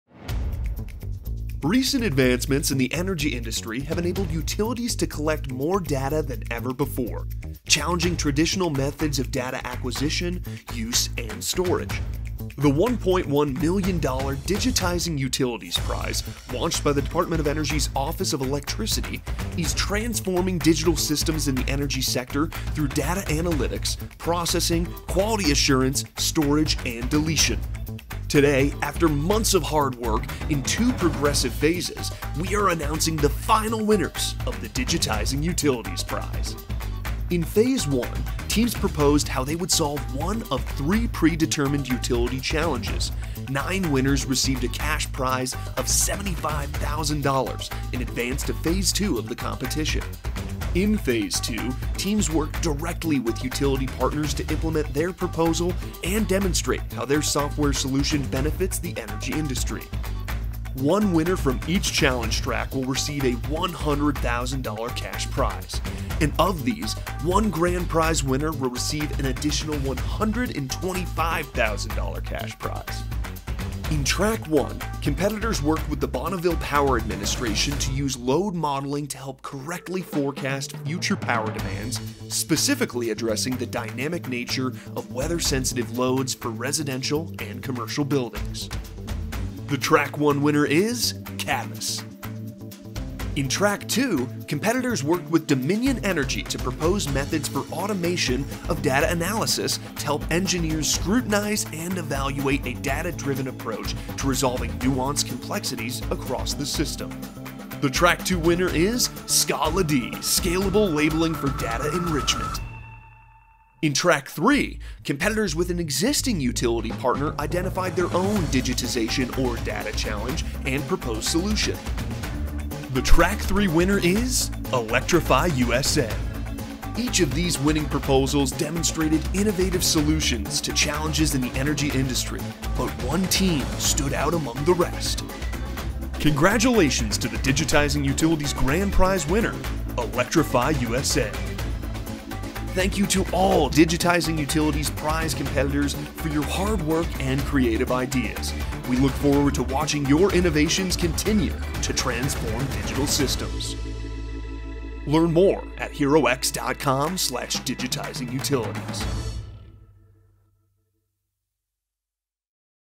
Digitizing Utilities Prize Phase 2 Winner Announcement
This video will reveal the three Phase 2 winners of the American-Made Digitizing Utilities Prize. Plus, find out who will take home the Grand Prize of $125,000. Learn more about the prize and the winning teams